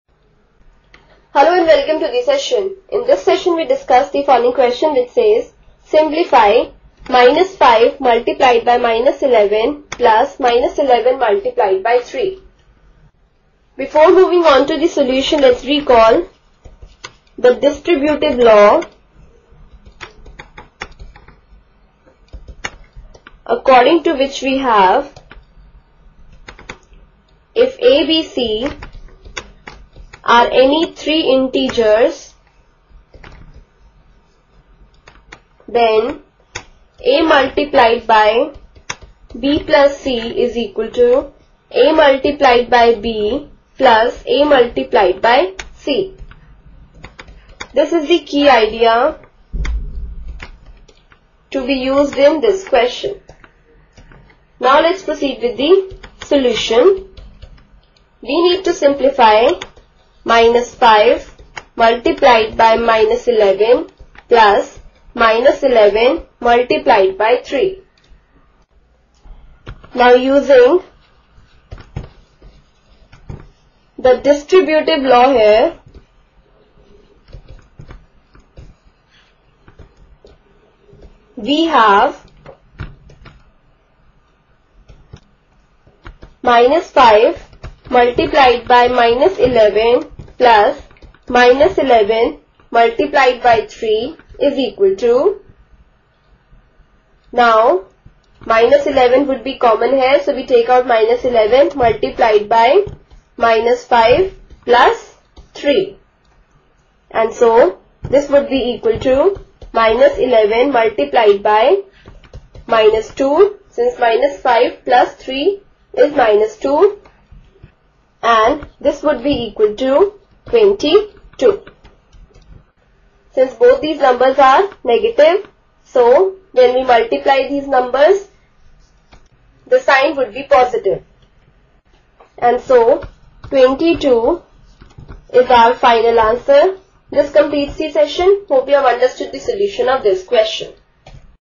Example: Simplify Using Distributive Law
In this example, we show how you can simplify an expression involving signed numbers using distributive law.